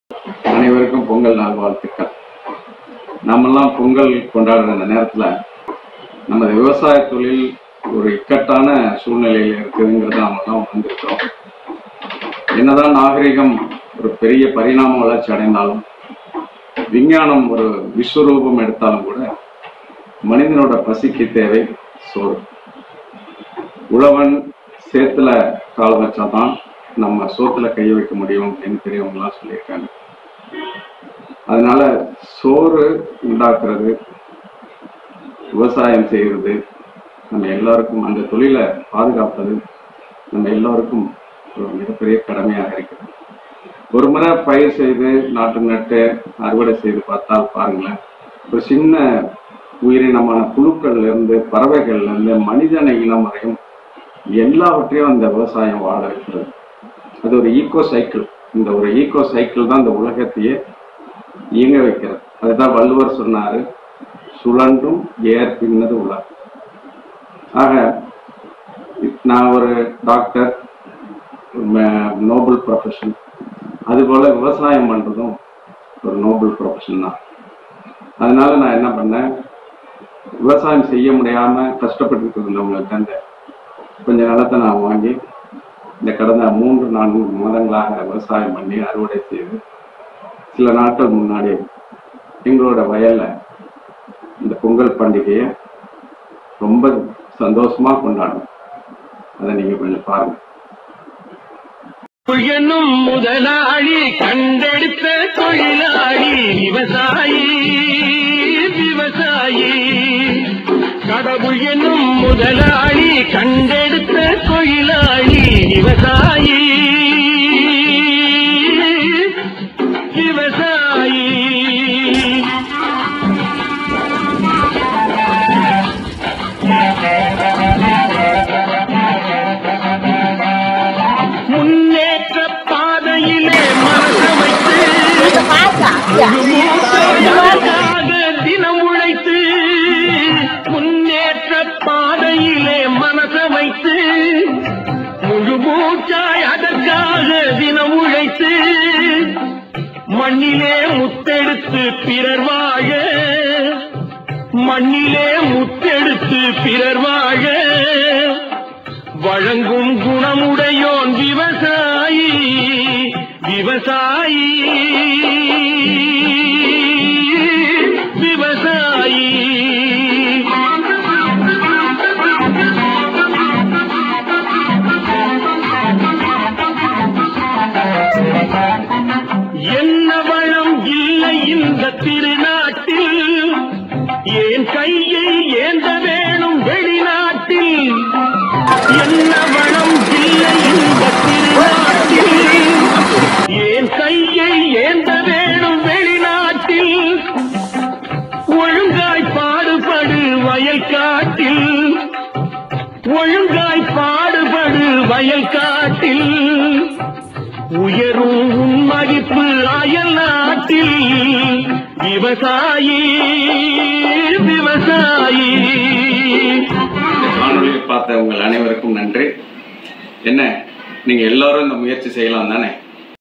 இனிய பொங்கல் நல்வாழ்த்துக்கள் # Happy Pongal 2019 - Agriculture Awareness
இனிய பொங்கல் நல்வாழ்த்துக்கள்

Agriculture Awareness Video by Dr KM Abul Hasan, Managing Director, City Hospital, Erode